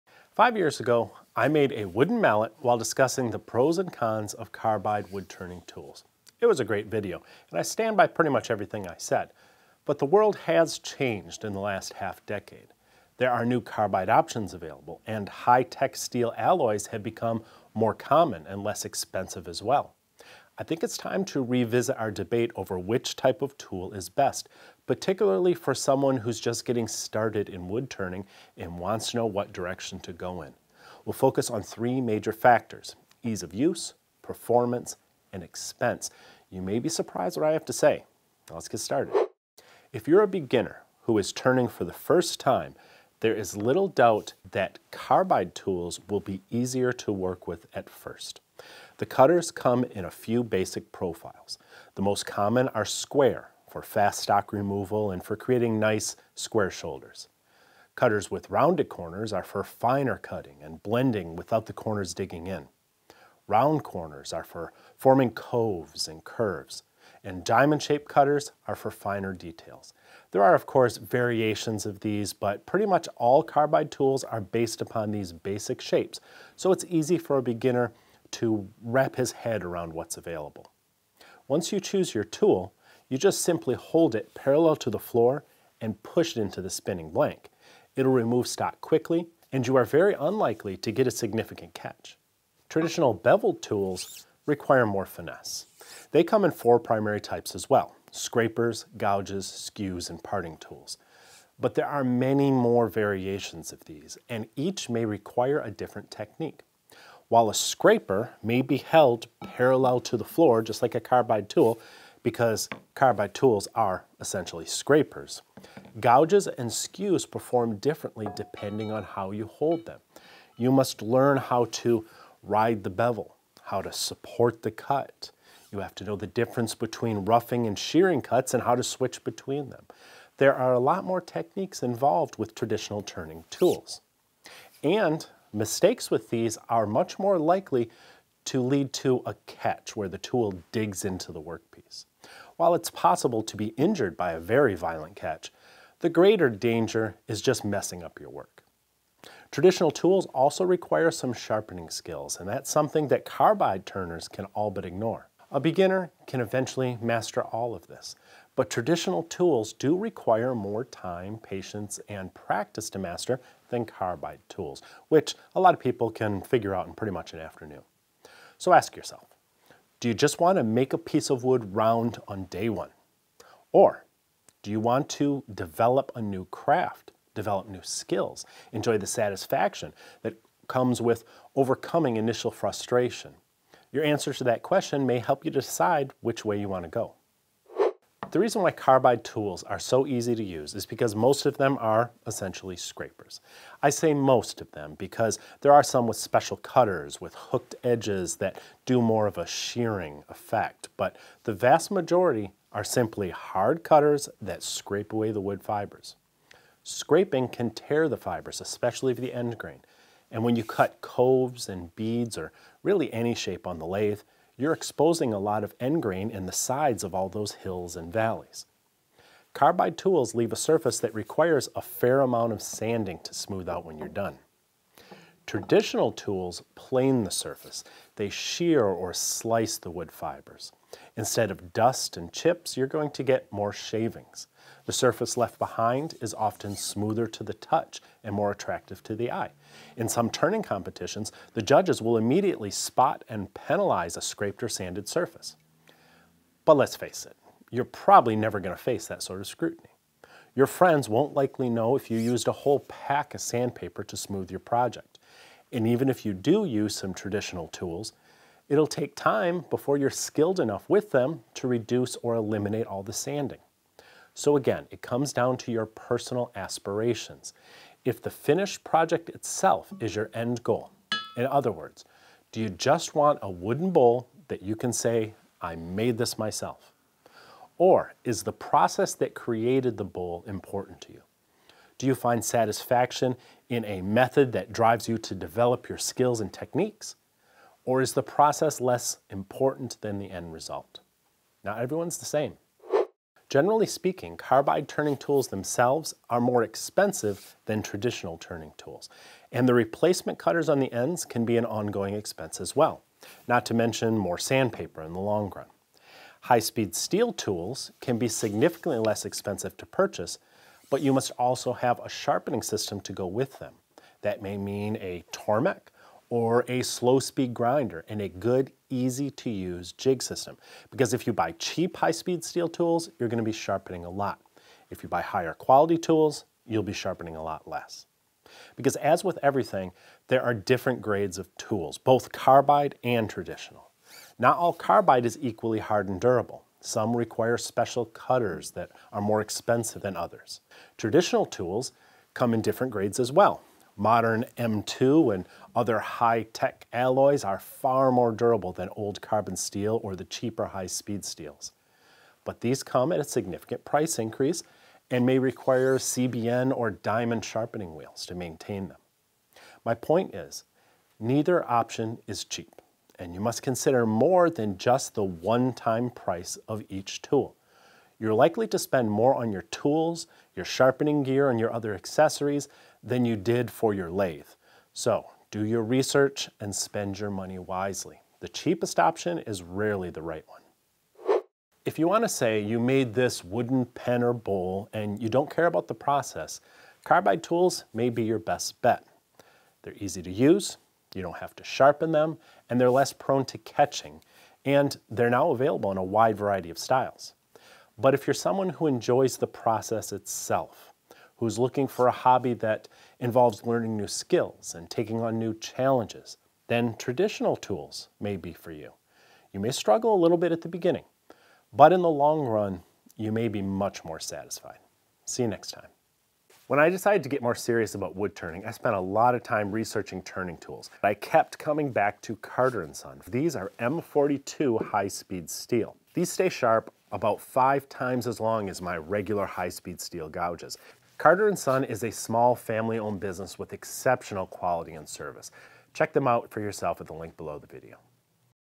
Watch this before buying carbide turning tools
Which turning tools should you get- Carbide or Steel?
▼EXPAND THIS SECTION FOR MORE RESOURCES RELATED TO THIS VIDEO▼